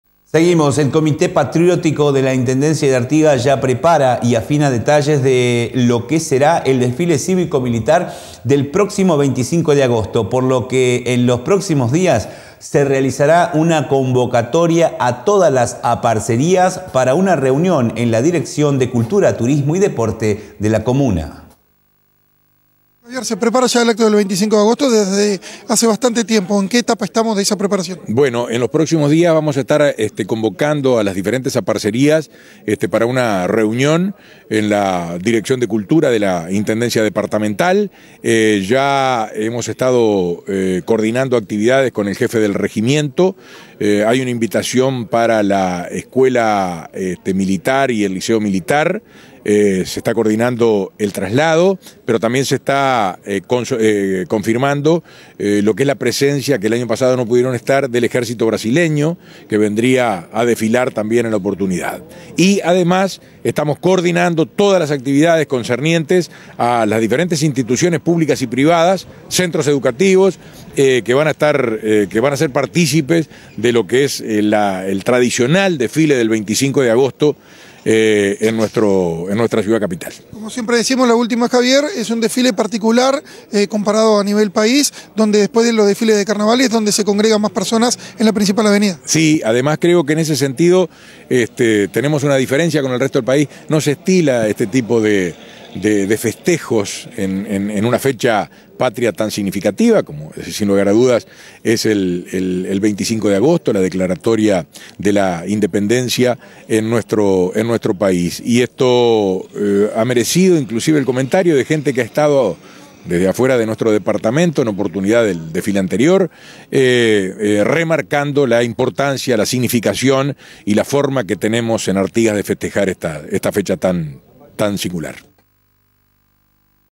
Infotres - 20/07/2023 - Javier Bertalot
El Comité Patriótico de la Intendencia de Artigas ya prepara y afina detalles de lo que será el desfile Cívico Militar del próximo 25 de Agosto, por lo que en los próximos días se realizará una convocatoria a todas las aparecerías para una reunión en la Dirección de Cultura, Turismo y Deporte de la Comuna.